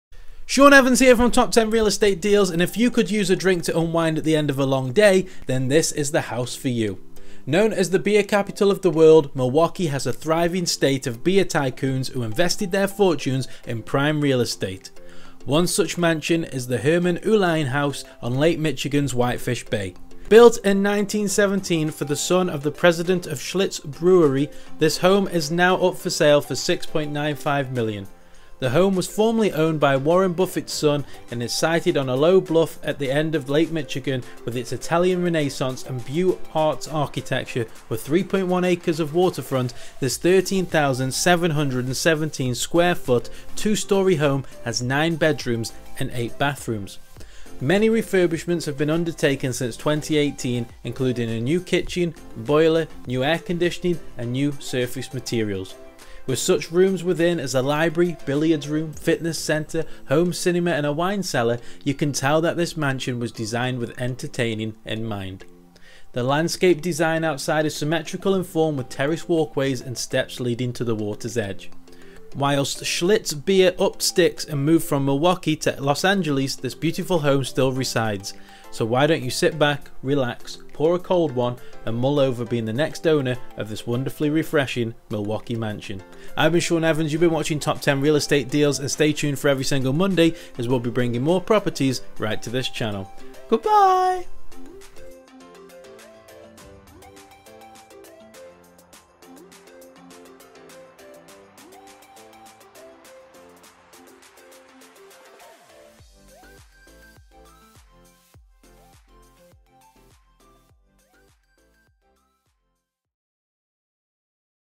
Inside the $6.95 Million Milwaukee Beer Mansion
The success of the Milwaukee beer tycoons was apparent in the mansions built from their fortunes such as the Herman Uihlein House on Lake Michigan’s Whitefish Bay built in 1917 for the son of the president of Schlitz Brewery. Before the home was built, the property was the site of the Pabst (Beer) Whitefish Bay Resort with a restaurant, round beer bar with lake views, and a Ferris wheel. The Herman Uihlein House is now for sale at $6.95 million.

Photo Credit: VRX Media Group and Rangeline Photography